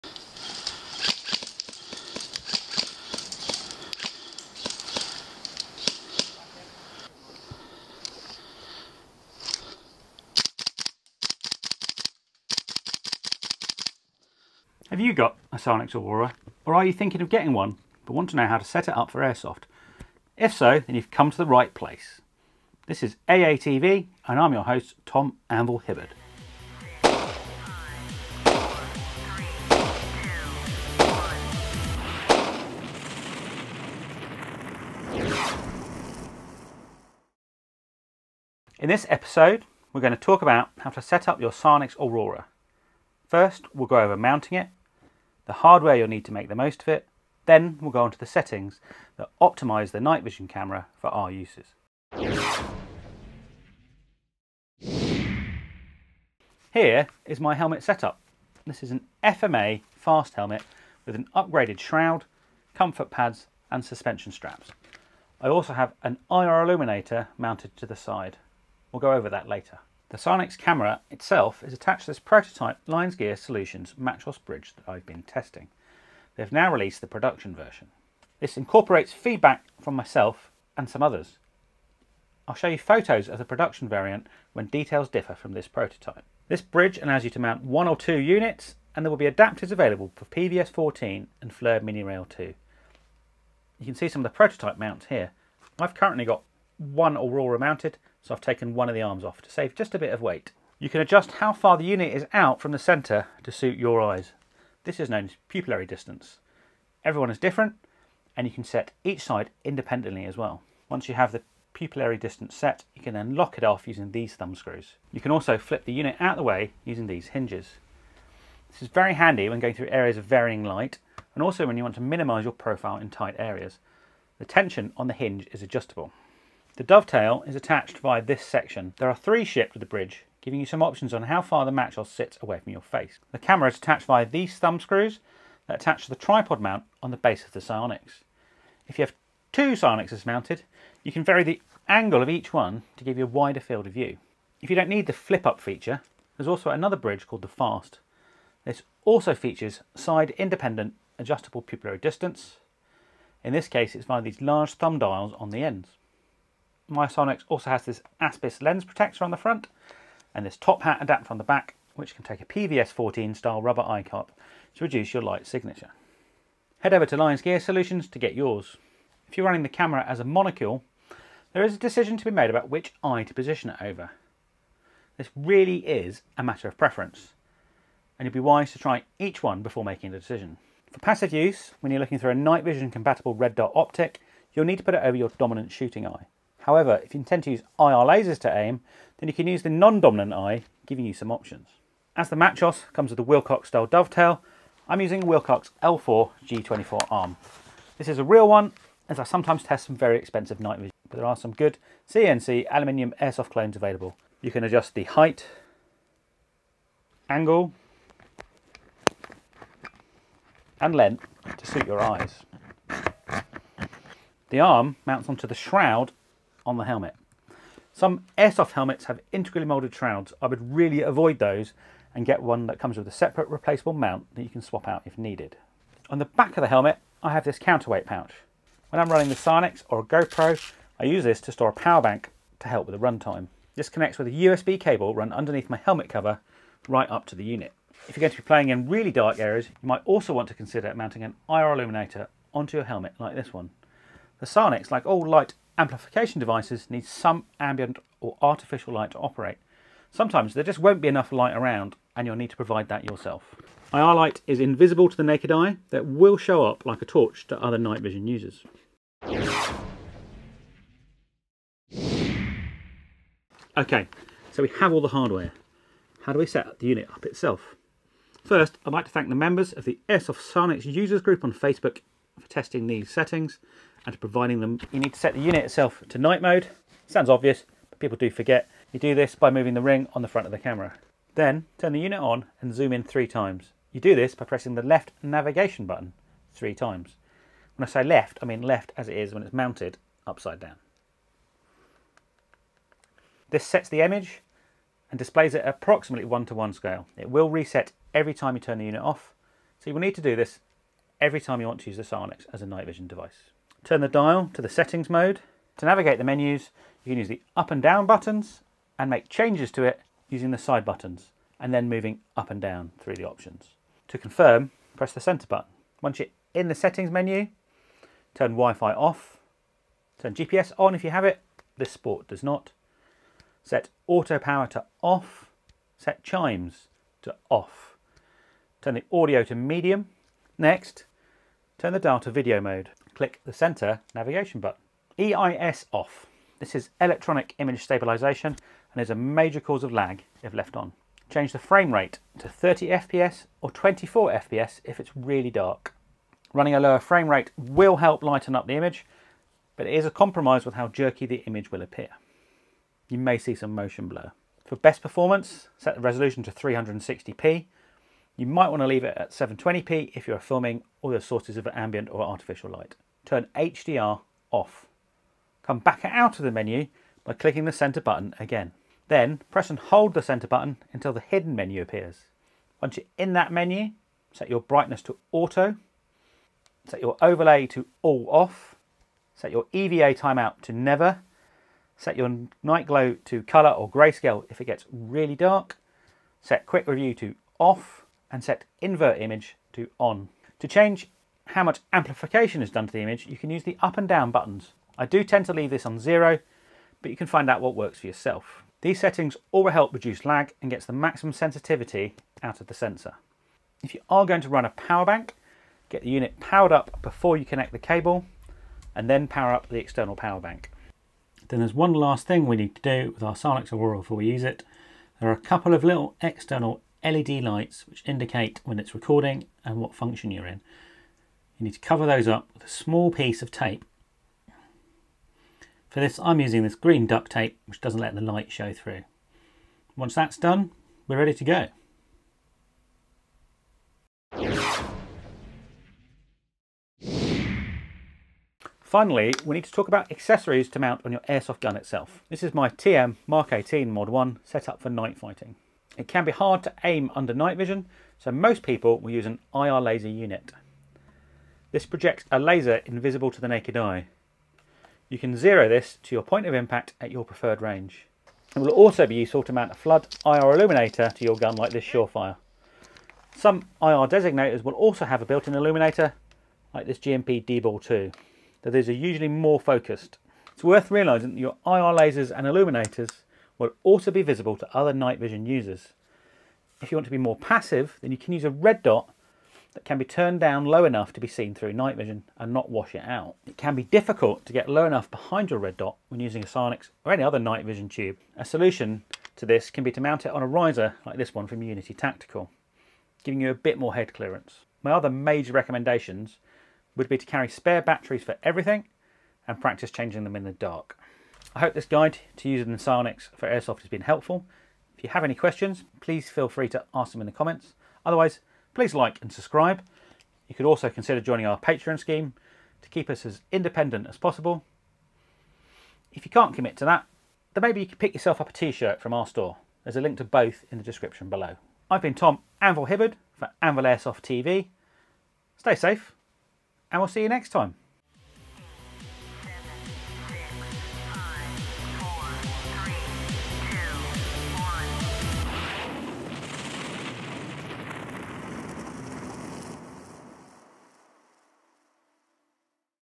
SiOnyx Airsoft Setup Guide | Best SiOnyx Helmet Mount | AATV EP135
Airsoft Guide to setting up and using the SiOnyx Aurora night vision camera as a Helmet Mounted device.

Chapters:
00:00 Start
00:38 Intro
00:54 Helmet Hardware
04:21 SiOnyx Settings
07:15 Airsoft Gun Setup
08:20 Outro

SiOnyx Helmet Mount Settings: Many thanks to the SiOnyx Airsoft Users Group on Facebook for developing these!
1. Set to night mode.
3. Turn the unit on and zoom in 3 times. You will need to do this every time you want to use the SiOnyx as a NightVision Device.

TURN THE DIAL TO THE SETTINGS MODE
1. Turn WiFi off
2. GPS on if you have it. The sport does not.
3. AUTO POWER - OFF
4. Chimes off.
5. Audio to medium
TURN THE DIAL TO THE VIDEO MODE
1. EIS off.
2. Frame rate to 30fps or 24fps if it’s really dark,
3. Resolution to 360p. You might want to leave it at 720p if you are filming.
4. HDR - OFF

Come out of the menu by clicking the centre button again.
PRESS AND HOLD the centre button until the hidden menu appears
1. Brightness to auto.
2. Overall all off.
3. EVF timeout to never
4. Night Glow to colour or Greyscale if it gets really dark.
5. Quick Review to OFF
6. Invert Image to ON

Helmet Setup as shown in the video. Some of this isn’t easily commercially available or in production.

FMA Fast Helmet.
Crye Airframe Helmet Suspension System
FMA Comfort Foam Pads
Wilcox L4 Shroud
Wilcox L4 G24 Arm
First Spear Stretch FAST Helmet Cover
FMA Counterweight Pouch
10,000 mAh Powerbank
FMA Helstar Clone
Lion’s Gear MACHOS V2020 Bridge
Fenix TK25 IR Torch
Magpul Rail Light Mount

Hosts:
Tom 'Anvil' Hibberd

Camera:
Jo Horsefield
Erin Flood

Production & Editing:
Black Dog Media